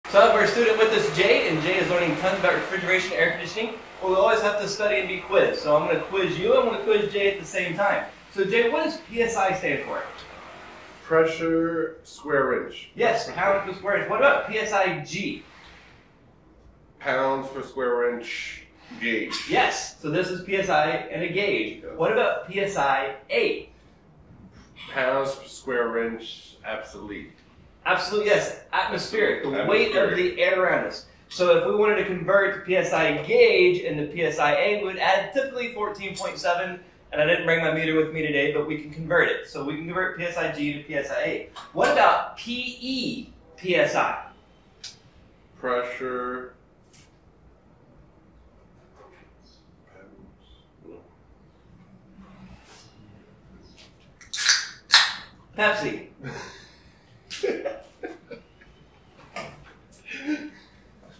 PSI, do you know it? pop quiz, bad audio warning
WARNING bad audio, the mic was not working
PSI Pounds per Square Inch
PSIG Pounds Per Square Inch Gauge
PSIA Pounds Per Square inch Absolute, including atmospheric
AND